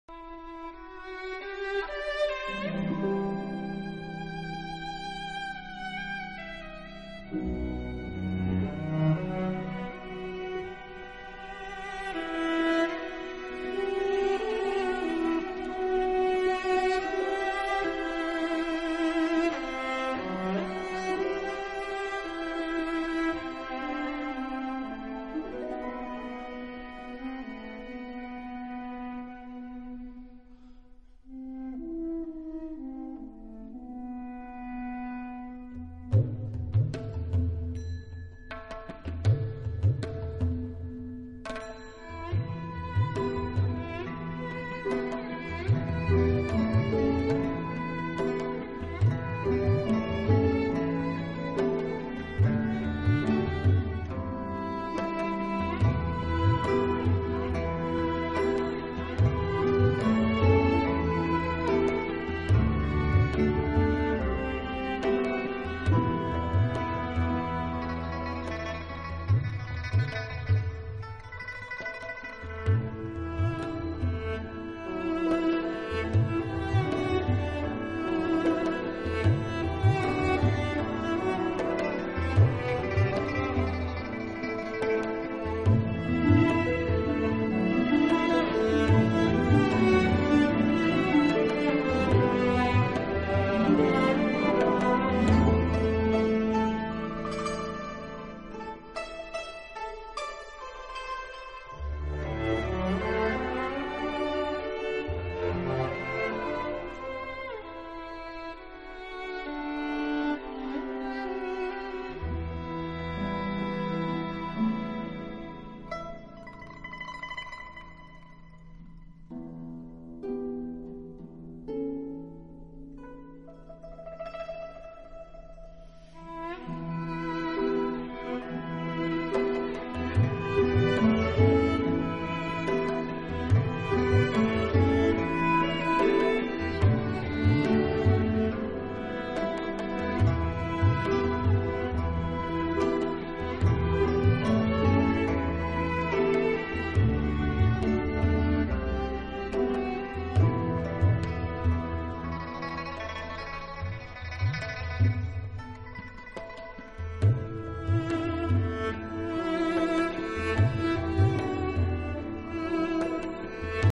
California landscape to Yo Yo Ma Music
photos of a hike taken in the Mt. Diablo east bay region of  northern California.